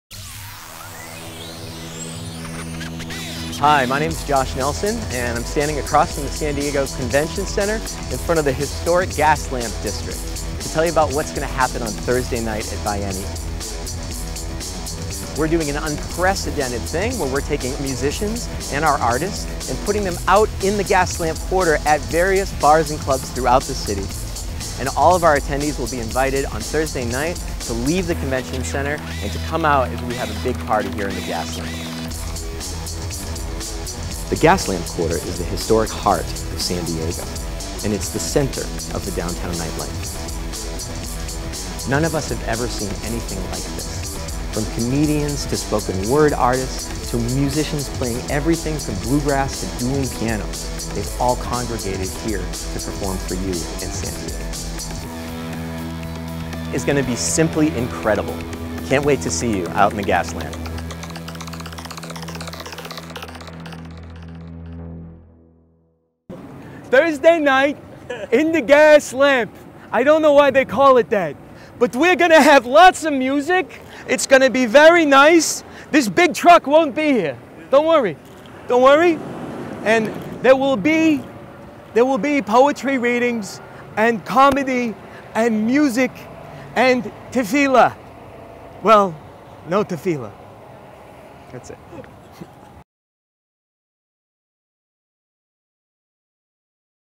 Biennial 2013 Gas Lamp Video by Josh Friedman Productions
Josh Nelson narrates and introduces Biennial 2013's Thursday night event in the Gas Lamp District.
Film and Music by Josh Friedman Productions